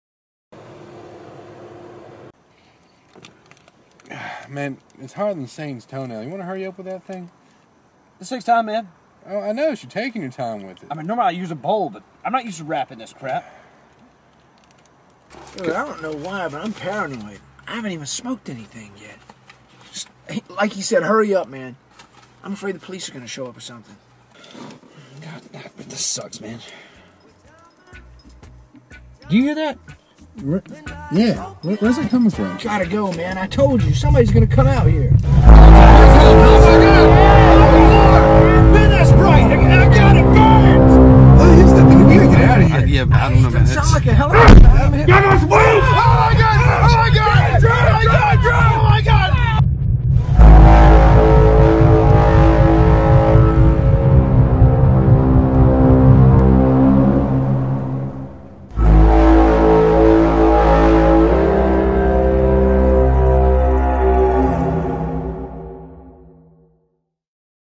Xenophobia
A group of friends drive out into the country to chill when some unexpected company shows up.